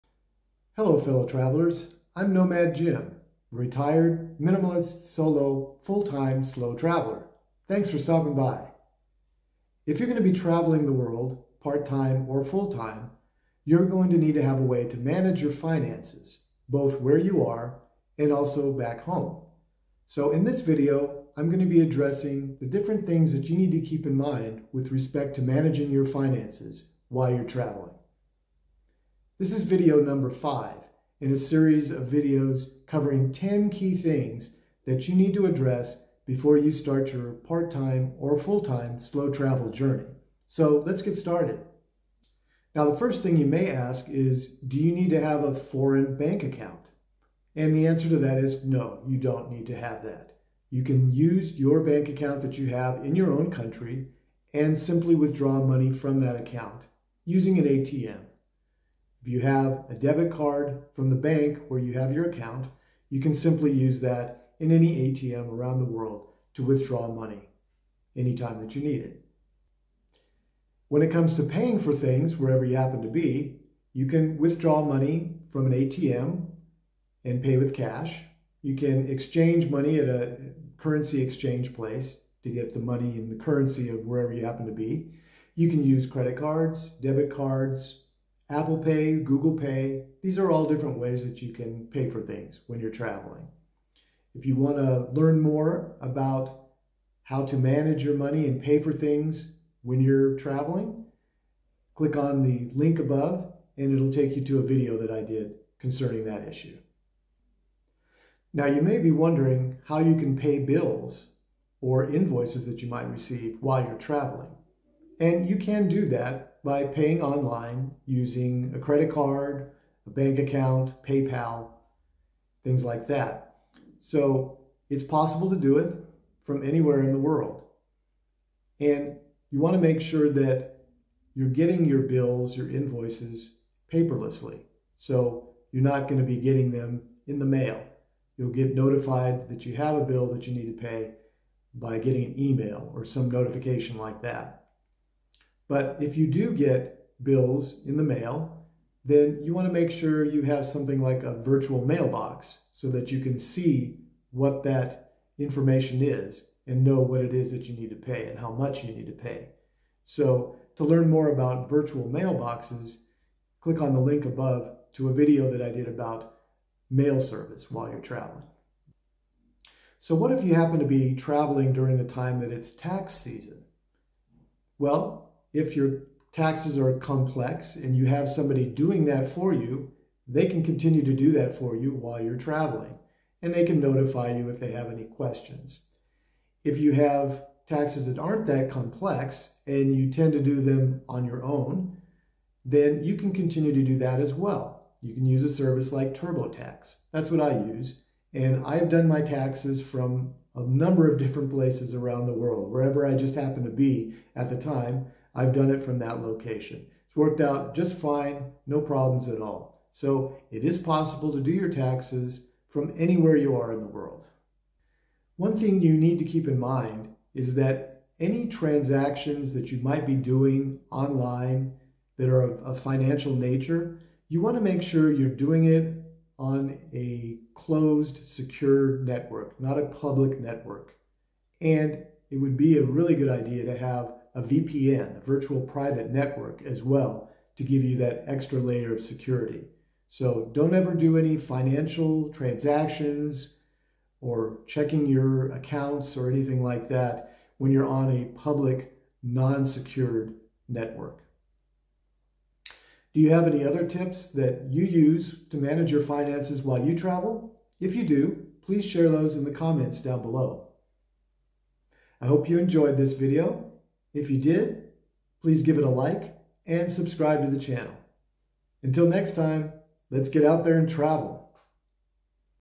Master Your Money: Travel Finance Tips You Need!
If you’re going to be traveling part-time or full-time, you’re going to want to make sure you’re able to stay on top of your finances, both where you are and back home, while you're traveling the globe. In this video I’ll be talking about the things you need to keep in mind with respect to managing your finances as you travel.

This is video #5 in a series that covers 10 key things to address before starting your part-time or full-time slow travel journey